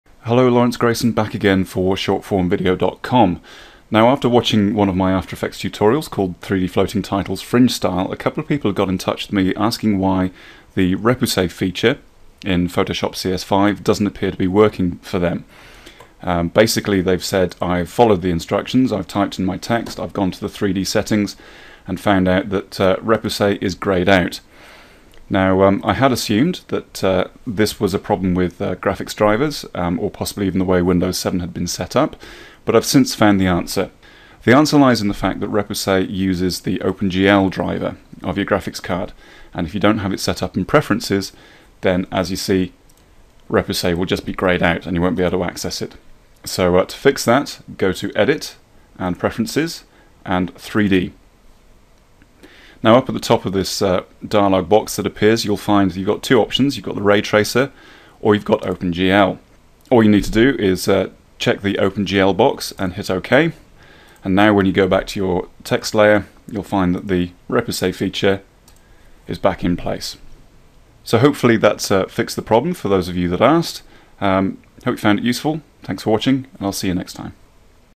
Photoshop CS5 - Repousse not working fix
A couple of people have asked me why Repousse isn't enabled in their installation of Photoshop CS5 when they follow my 3D floating titles tute.
You need to enable OpenGL in your preferences - so here's a video that shows you how.